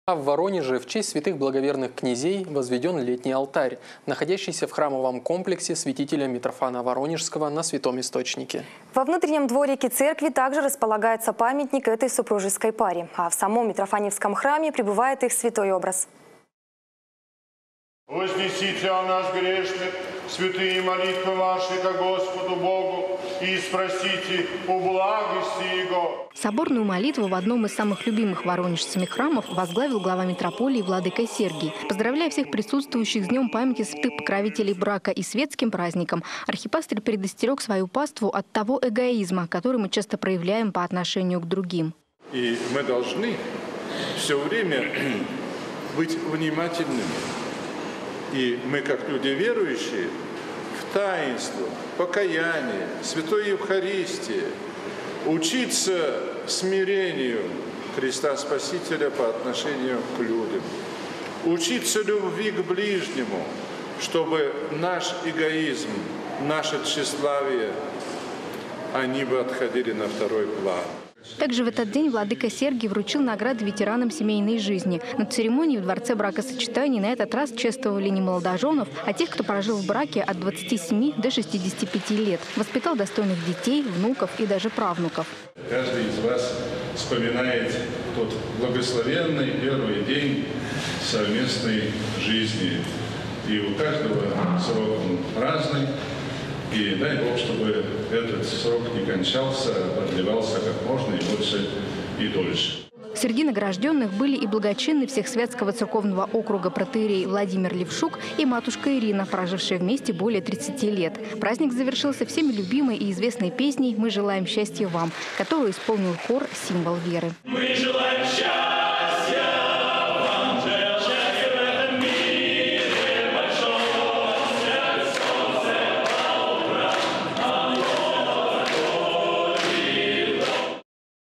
В Воронеже прошли праздничные мероприятия в честь Дня семьи, любви и верности
В Воронеже в честь святых благоверных князей Петра и Февронии возведен летний алтарь, находящийся в храмовом комплексе святителя Митрофана Воронежского на святом источнике. Во внутреннем дворике церкви также располагается памятник этой супружеской паре, а в самом Митрофаньевском храме пребывает их святой образ.